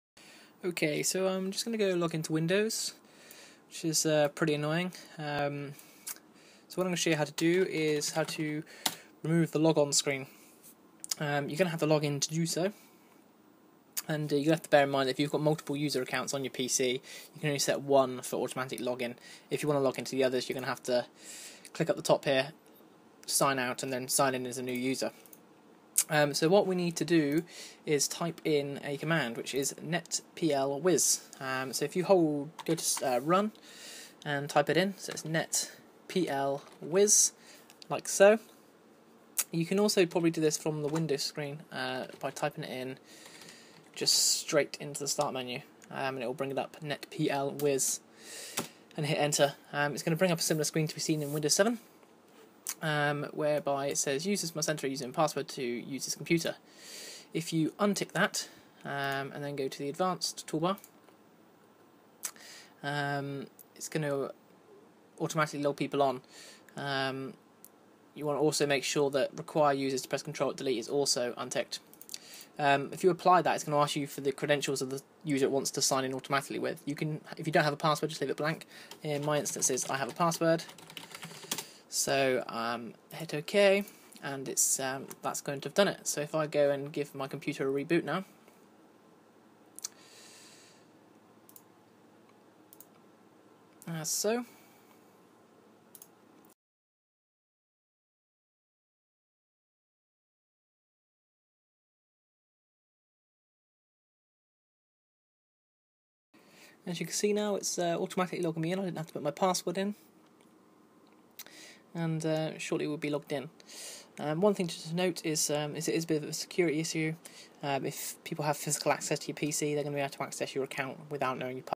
Remove Logon Screen Windows 8.1 [HD][How To][Step by Step Guide][Tutorial] 2023
How to remove the logon screen on boot from Windows 8 and have Windows logon automatically for you. This will let you have quicker access to your files and applications. Always be aware of the security risk of automatic access to your files. You also wont need to press CTRL+ALT+DEL